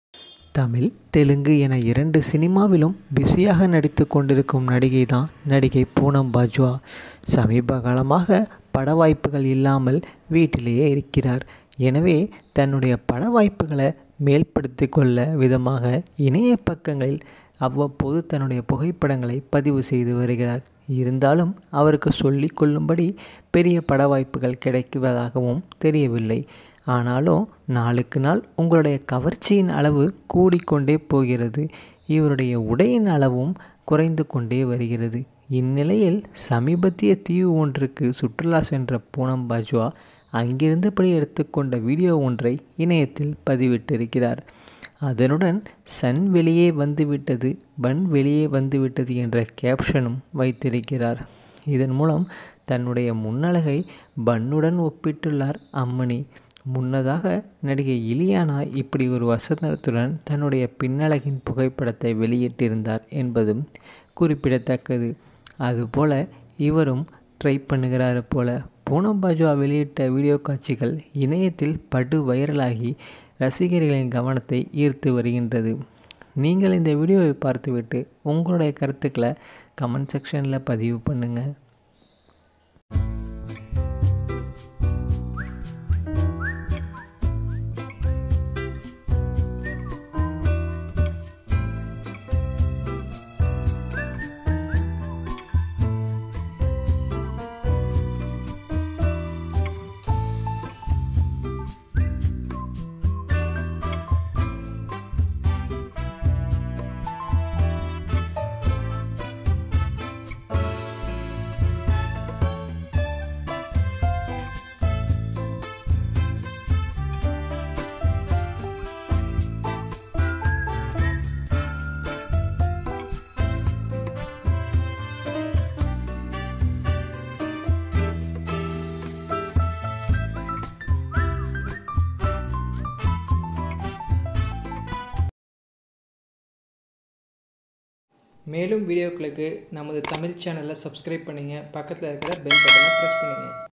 தன்னுடைய முன்னழகை உணவு பொருளுடன் ஒப்பிட்ட பூனம் பாஜ்வா..! – என்ன பொருள்-ன்னு பாருங்க..!
We talk about tamil actress poonam bajiwa trending photoshoot videos.